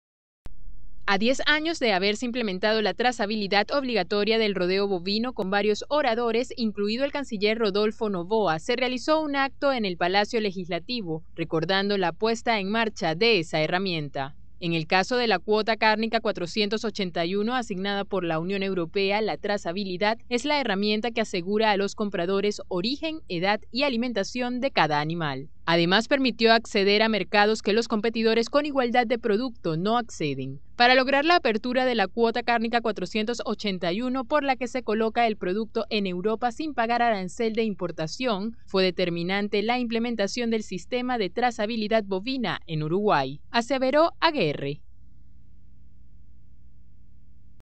Cuota 481 generó cerca de 300 millones de dólares a Uruguay
La trazabilidad obligatoria de todo el rodeo bovino, herramienta que rige desde septiembre de 2006, permitió que ingresaran a Uruguay entre 250 y 300 millones de dólares a través de las 50.000 toneladas de carne bovina de alta calidad, en el marco de la cuota 481, donde se logró un precio promedio de 9.163 dólares.